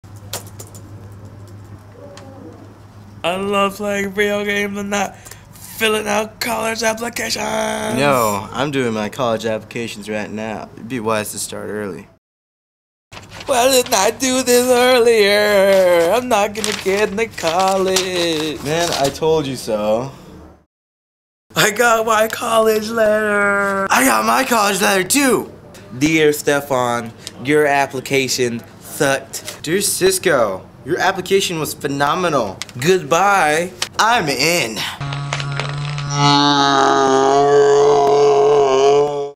College Applications Commercial for SCNN
A video made for Shorecrest's SCNN that gives reason to starting to work on your college applications early.
Cast (in order of appearance): Stefan, Sisco.
Director/Editor: Will Greenburg